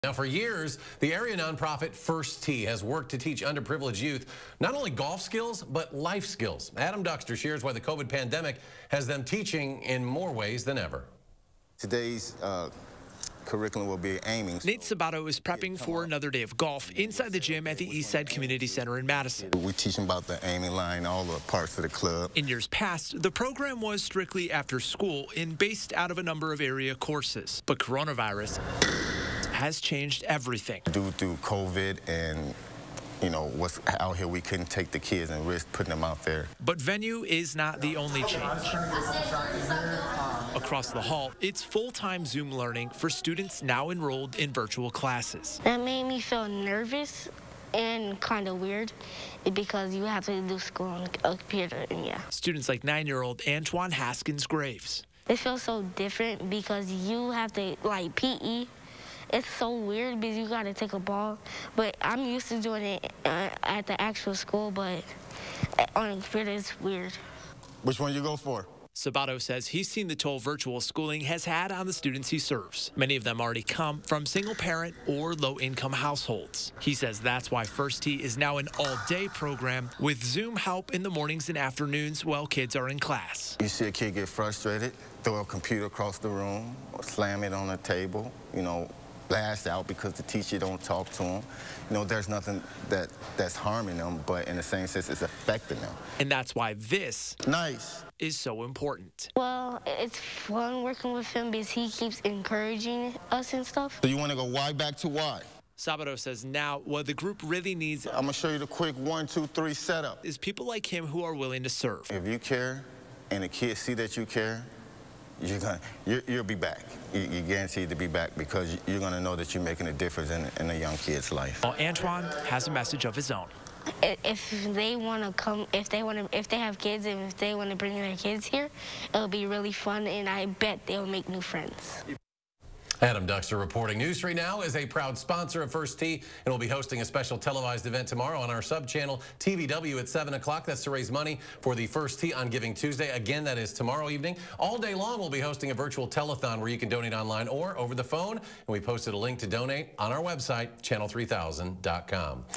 'They need time to be kids': Area non-profit First Tee works to fill gaps created by online
The South-Central Wisconsin Branch of the First Tee Organization says it's trying to fill the gaps for students dealing with online classes and isolation linked to the COVID-19 pandemic.